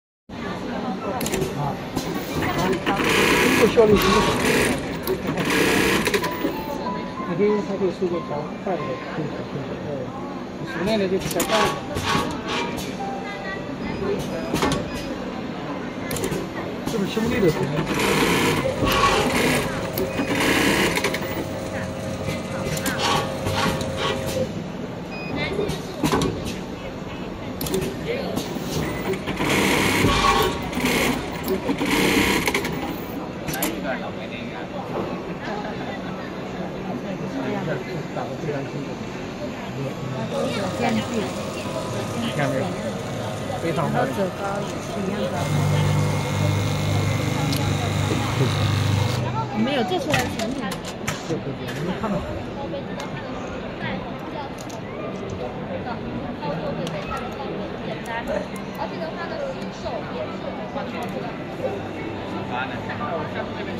Automatic curtain pleating machine
This is the operation demonstration of the fully automatic curtain pleating machine, which can save costs and improve production efficiency